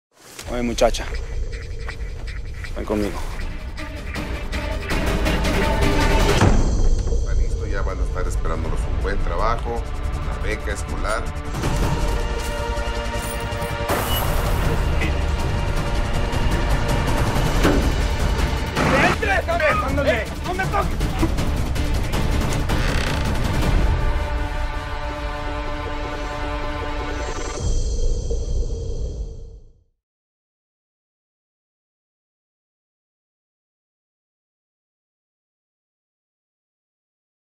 Contra la trata de personas y la explotación sexual
La trata de personas existe, si ves algo sospechoso, llama al: 9-1-1 o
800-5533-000 Línea  Nacional Contra la Trata de Personas